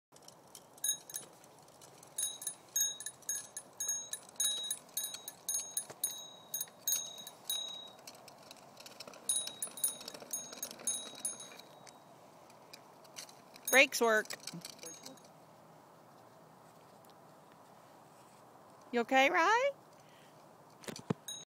Ry gets a big boy bike
Finally ! A big boy bike at Papaw & Gigi's house!